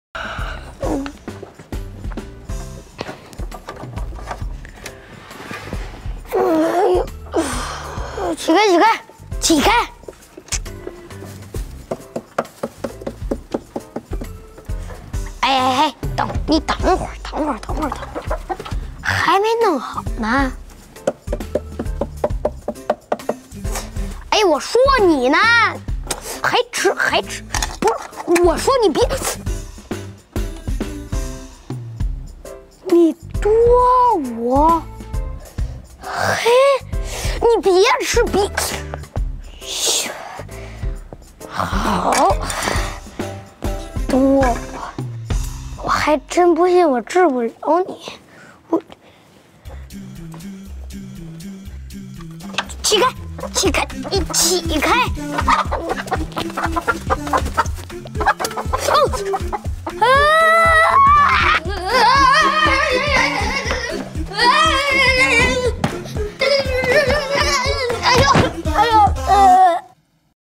Một vai diễn đáng yêu của Hồ Tiên Hú khi còn nhỏ | 胡先煦小时候
Lúc trước m up lỗi. Mình up lại :"
Xem đi xem lại bao nhiêu lần vẫn thích cảnh này. Cơ mặt linh hoạt, đáng yêu quá em ơi!!!